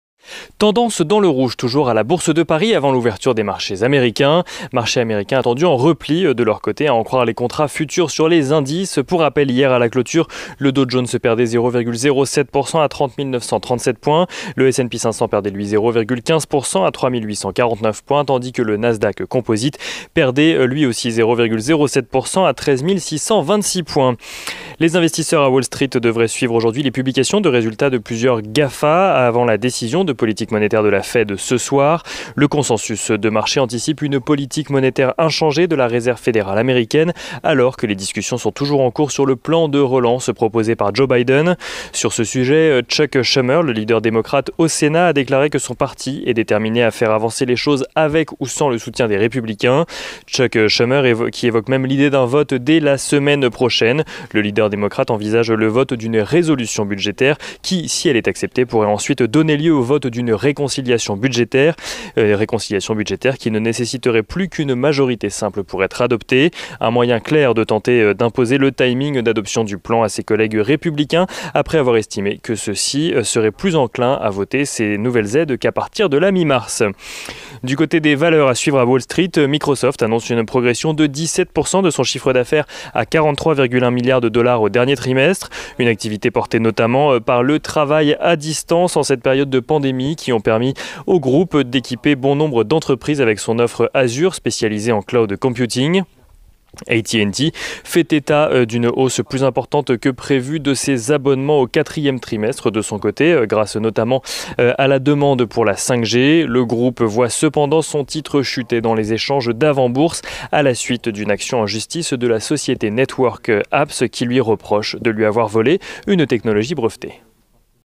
27/01 - 15h : Les marchés US attendus dans le rouge
Tendance dans le rouge toujours là la bourse de paris avant l’ouverture des marchés américains

Marché américains attendus en repli de leur côté à en croire les contrats futures sur les indices

Pour rappel :
DOW JONES : -0.07% à 30 937 points
S&P 500 : -0.15% à 3 849 points
NASDAQ : -0.07% à 13 626 points

Les investisseurs à Wall Street devraient suivre les publications de résultats de plusieurs GAFA aujourd’hui avant la décision de politique monétaire de la FED ce soir.

Le consensus de marché anticipe une politique monétaire inchangée de la Réserve Fédérale américaine alors que les discussions sont en cours sur le plan de relance proposé par Joe Biden.

Sur ce sujet, Chuck Schumer, le leader démocrate au Sénat a déclaré que son parti est déterminé à faire avancer les choses avec ou sans soutien des Républicains. Chuck Schumer qui évoque même l’idée d’un vote dès la semaine prochaine. Le leader démocrate envisage le vote d’une « résolution budgétaire » qui si elle est acceptée pourrait ensuite donner lieu au vote d’une « réconciliation budgétaire » qui ne nécessiterait plus qu’une majorité simple pour être adoptée. Un moyen clair de tenter d’imposer le timing d’adoption du plan à ses collègues Républicains après avoir estimé que ceux-ci seraient plus enclin à voter ces nouvelles aides à la mi-mars.

Côté valeurs,
Microsoft annonce une progression de 17% de son chiffre d’affaires à 43.1 milliards de dollars au dernier trimestre. Une activité portée notamment par le travail à distance en cette période de pandémie qui lui ont permis d’équiper bon nombre d’entreprise avec son offre Azur spécialisée en cloud computing.

Le groupe voit cependant son titre chuter à la suite d’une action en justice de la société Network Apps qui lui reproche de lui avoir volé une technologie brevetée.